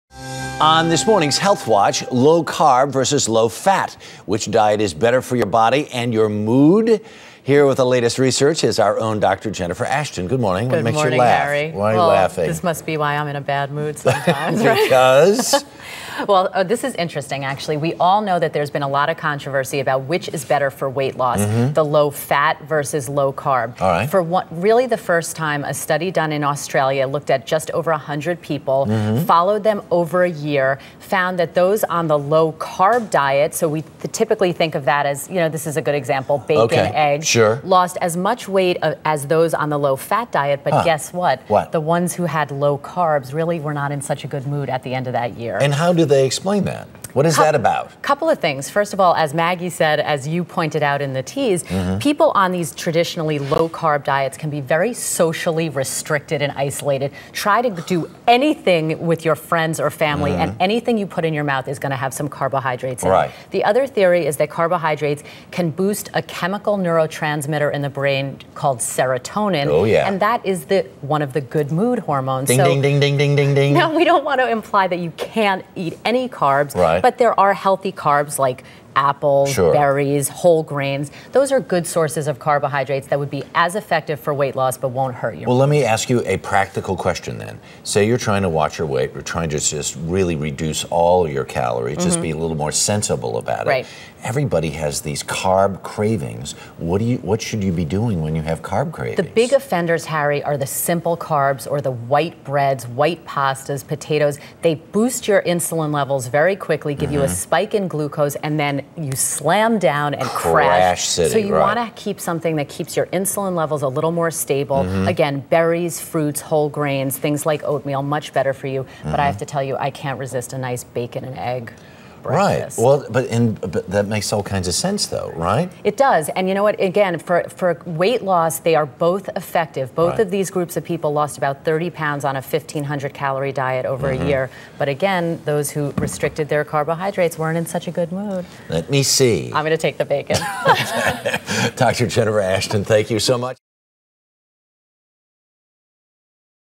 Low-Carb vs. Low-Fat Diets
Dr. Jennifer Ashton spoke to Harry Smith about the results of a study comparing low-carbohydrate versus low-fat diets, and which diet is best for your overall mood.